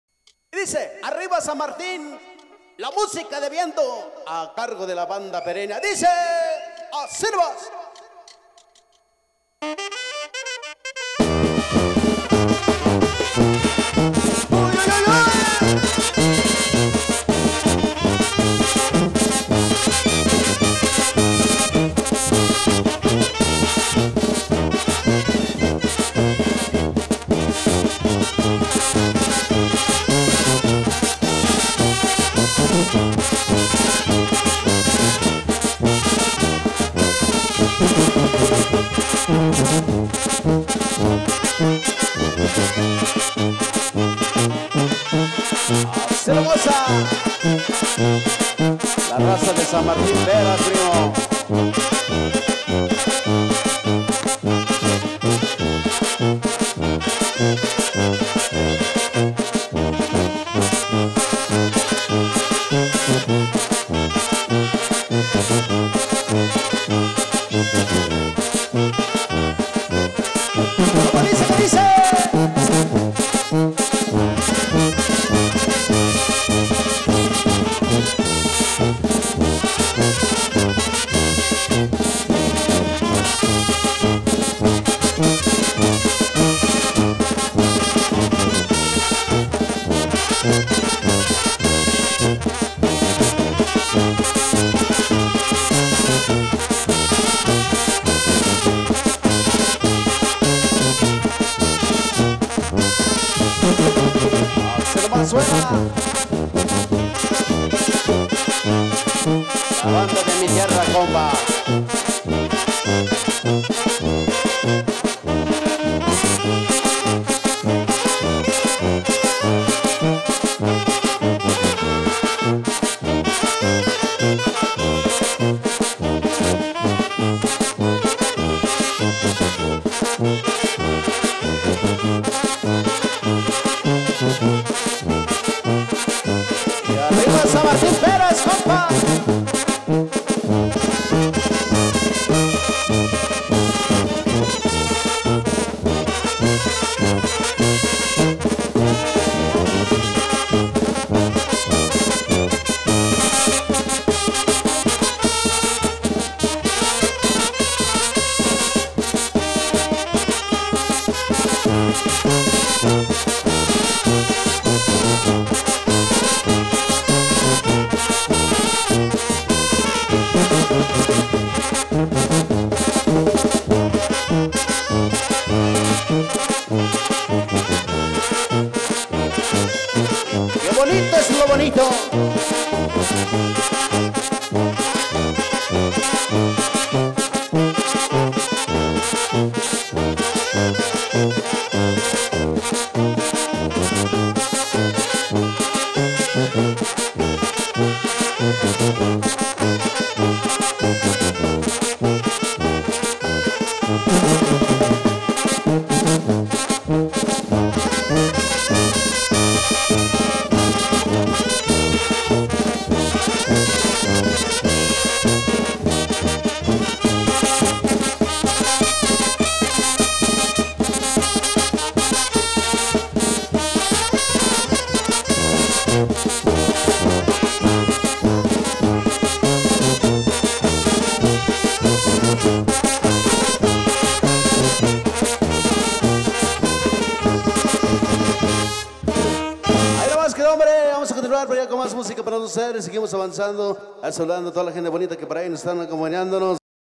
BANDA PEREÑA - Pista 5 (en vivo) jarabe
musica grabada en vivo. desde  un baile san martin peras, festejando el dia del padre.